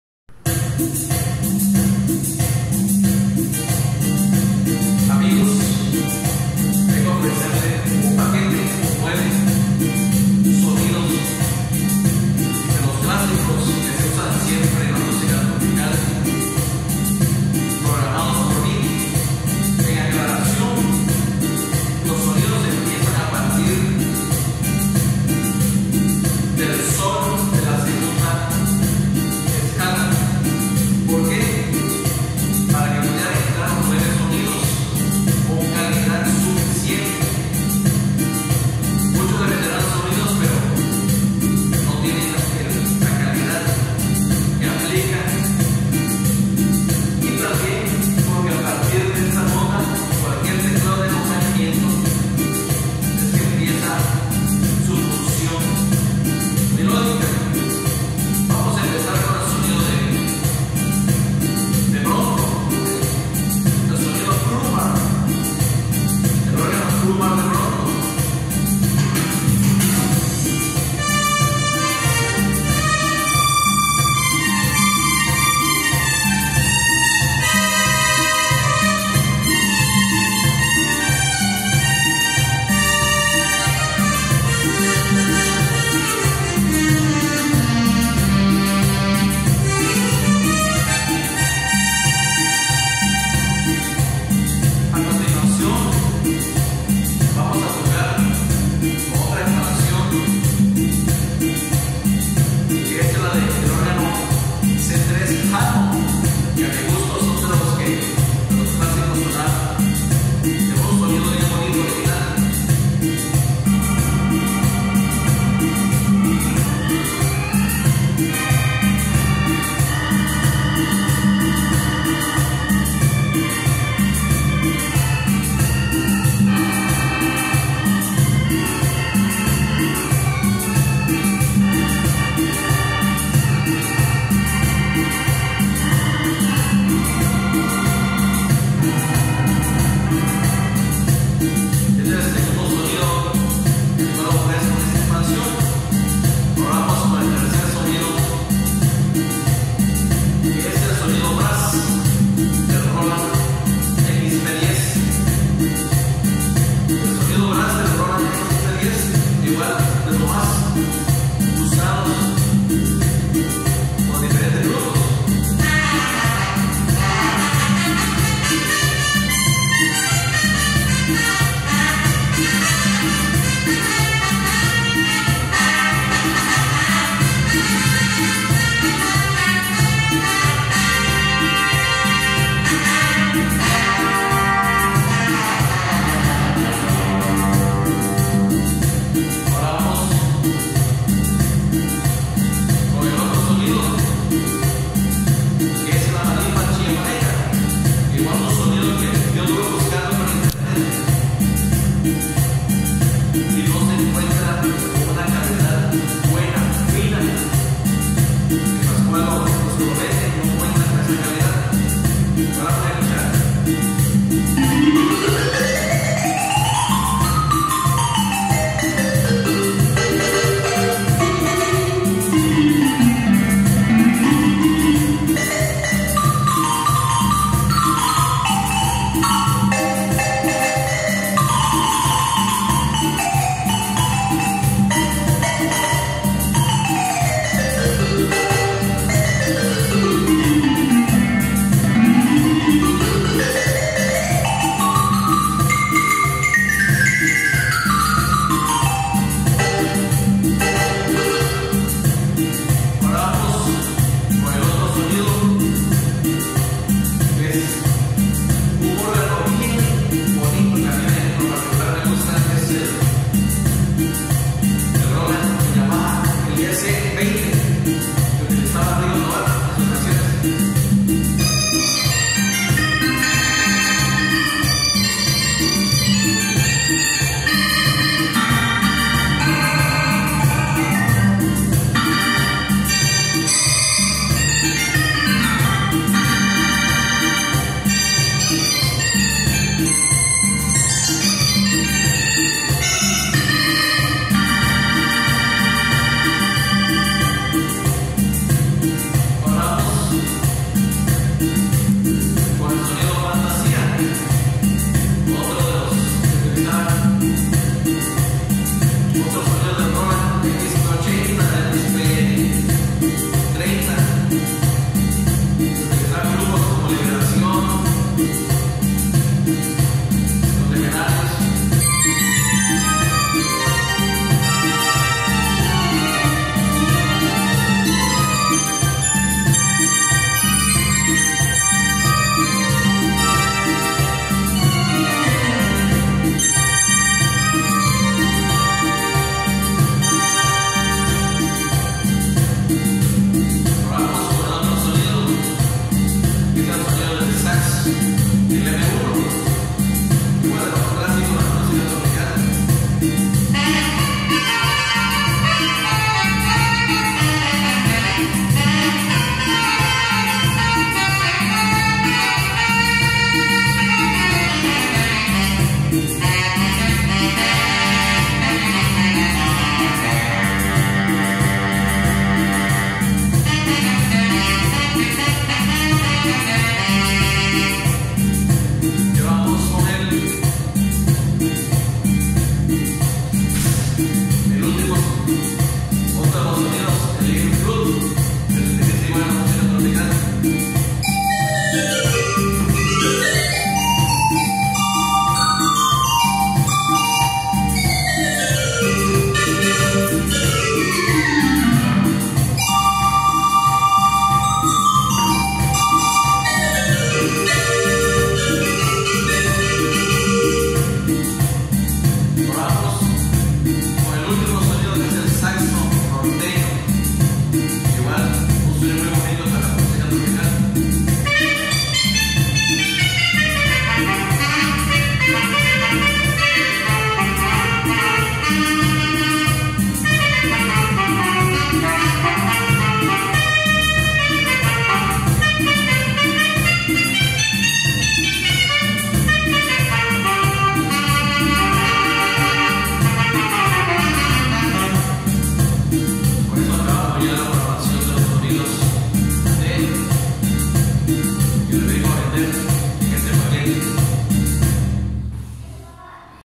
Mizuki Hirukawa Paquete1 en expansiones en venta .yep Para teclados de la serie Yamaha psr s
Demostración de sonidos en venta del paquete1 con 9 sonidos de expansiones .yep  que son usados en algunos modelos de teclado de la serie Yamaha Psr S
El precio de la expansión aqui descrita en este video es de 100 pesos moneda mexicana.